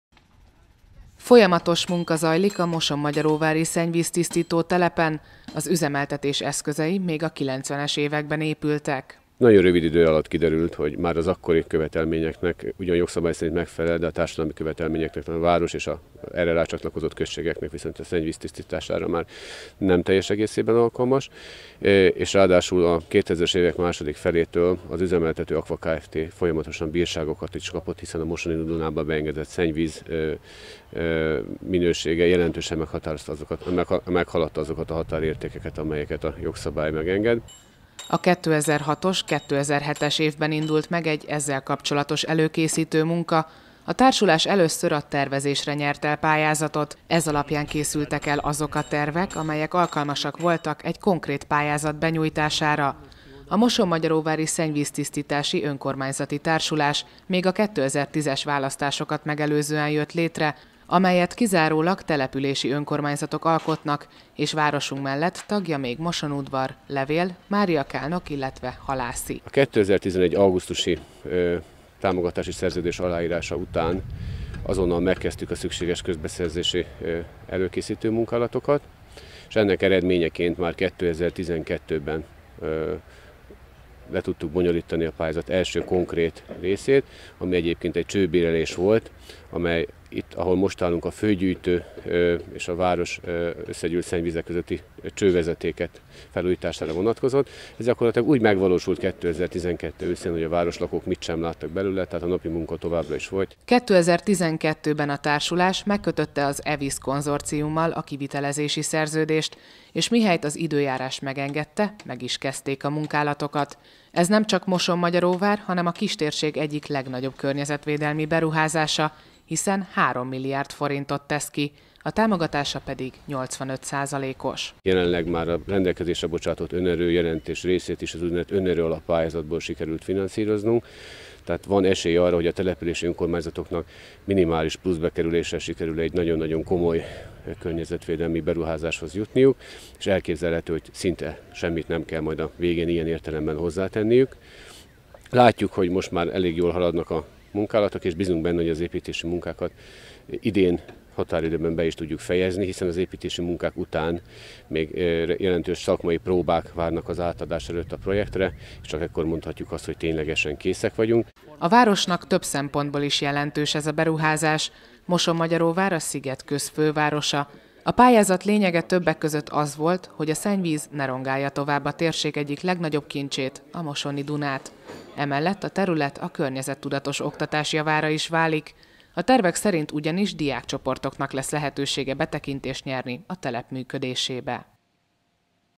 Szennyvíztisztító telep intenzifikáció_MVTV
Még a '90-es években épült a mosonmagyaróvári szennyvíztisztító telep, amelyet az előző évben megkötött szerződésnek megfelelően felújítanak. Az eszközfejlesztéseknek köszönhetően a végtermék kevesebb káros anyagot tartalmaz majd, mint azelőtt. A munkálatok nem befolyásolják a folyamatos és megfelelő üzemeltetést és a természetesen a lakók javát szolgálják.